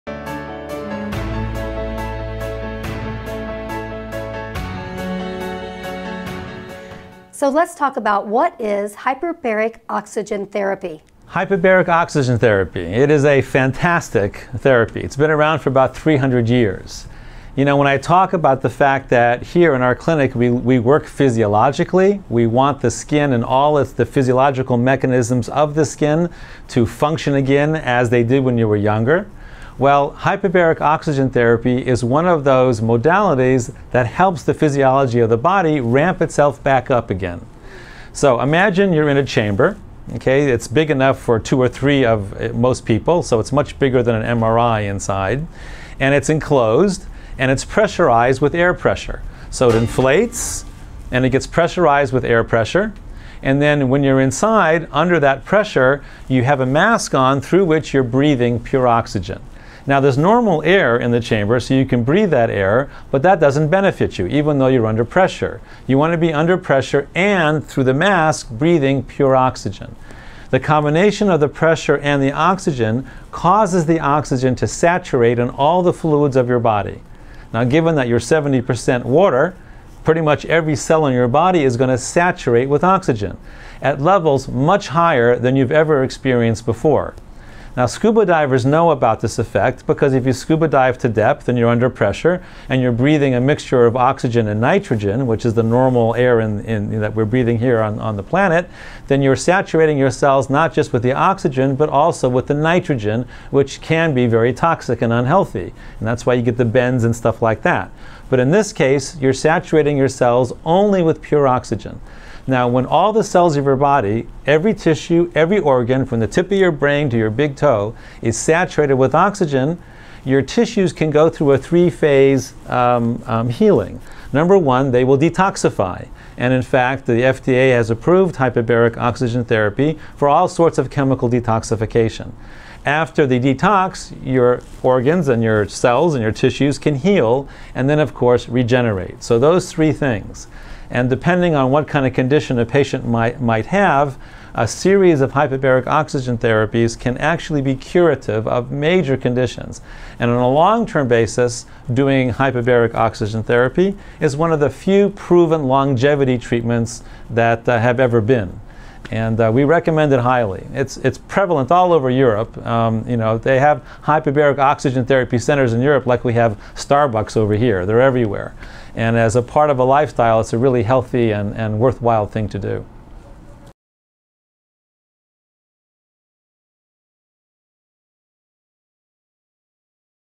What is Hyperbaric Oxygen Therapy or HBOT?
What is hyperbaric oxygen therapy? It is a fantastic oxygen therapy. It's been around for about 300 years. You know, when I talk about the fact that here in our clinic, we work physiologically, we want the skin and all of the physiological mechanisms of the skin to function again as they do when you were younger. Well, hyperbaric oxygen therapy is one of those modalities that helps the physiology of the body ramp itself back up again. So imagine you're in a hyperbaric chamber, it's big enough for two or three of most people. So it's much bigger than an MRI inside. And it's enclosed and it's pressurized with the air pressure. So it inflates, and it gets pressurized with air pressure and then when you're inside under that pressure, you have a mask on through which you're breathing pure oxygen. There is normal air in the chamber so you can breathe that air but that doesn’t benefit you even though you're under pressure. You want to be under pressure in the hyperbaric chamber and through the mask breathing pure oxygen. The combination of the pressure and the oxygen inside the chamber causes the oxygen to saturate in all of the fluids of your body.

Now when all the cells of your body, every tissue, every organ from the tip of your brain to your big toe is saturated with oxygen, your tissues can go to a three phase healing. Number one, they will detoxify and in fact, the FDA has approved hyperbaric oxygen therapy or HBOT for all sorts of chemical detoxification. After the detox, your organs and your cells and your tissues can heal and then of course regenerates; so those three things. And depending on what kind of condition the patient might have, a series of hyperbaric oxygen therapies can actually be curative of major conditions. And in a long terms basis, doing hyperbaric oxygen therapy is one of the few proven longevity treatments that they have ever been and we recommend it highly. It's prevalent all over Europe, they have hyperbaric oxygen therapy centers in Europe like we have Starbucks over here, they're everywhere and as a part of a lifestyle, HBOT is a really healthy and worthwhile thing to do.

AMA Regenerative Medicine & Skincare
Alice Pien, MD
Asher Milgrom, PhD

Orange County / South Coast Metro

Beverly Hills / Los Angeles
6310 San Vicente Blvd., Suite 285

BREATHE IN! HYPERBARIC OXYGEN THERAPY – WHAT YOU NEED TO KNOW